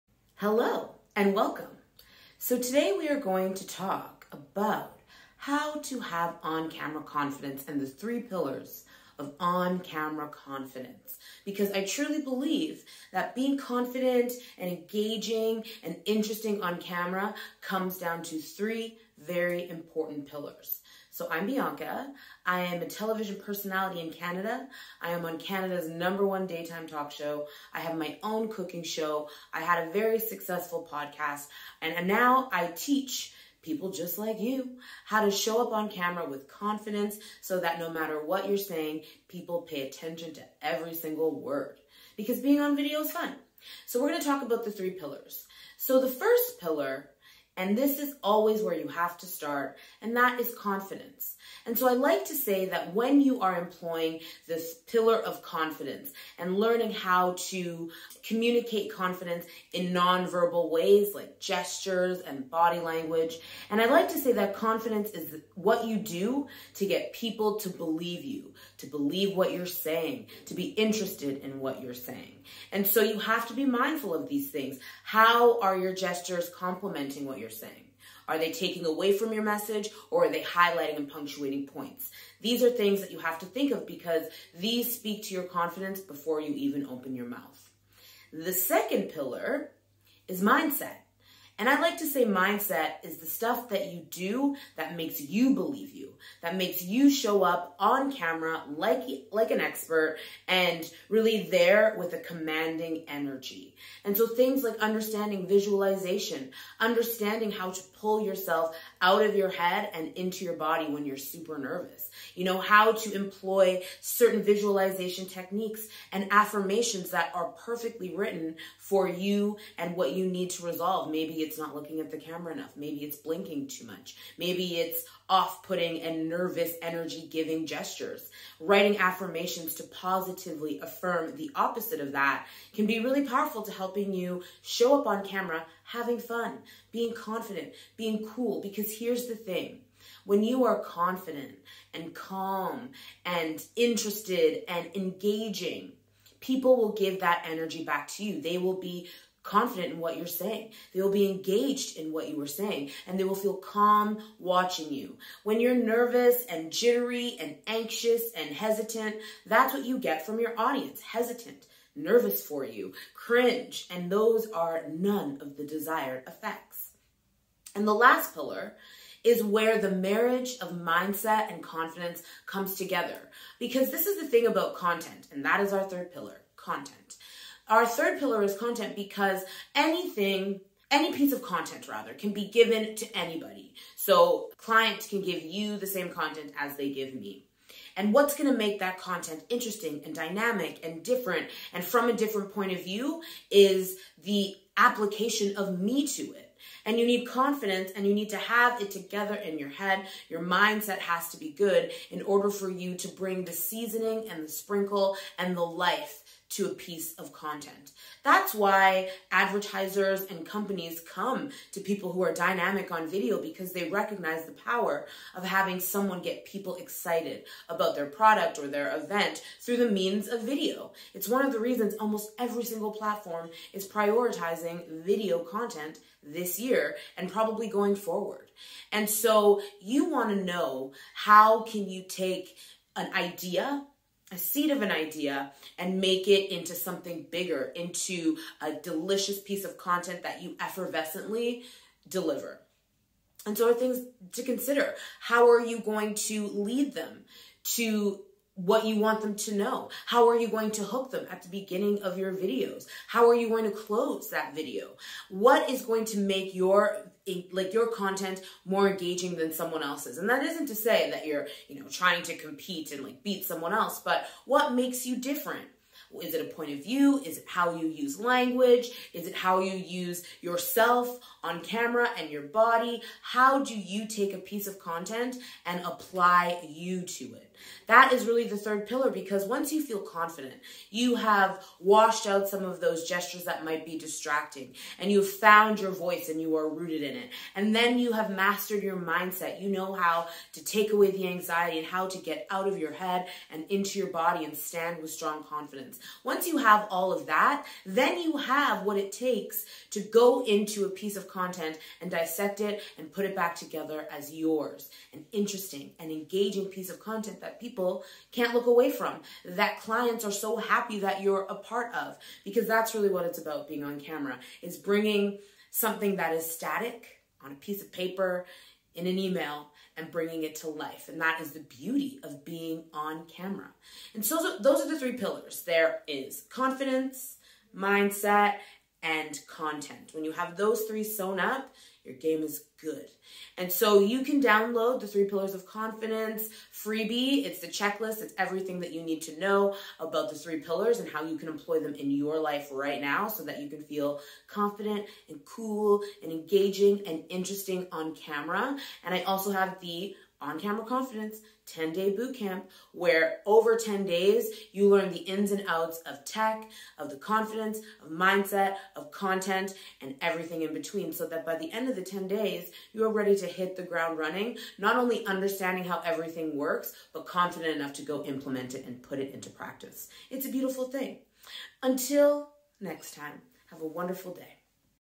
HOW TO FEEL COMFORTABLE ON CAMERA
In this video learn from TV personality and on camera confidence coach Bianca Osbourne, the 3 Pillars of Feeling Comfortable on Camera!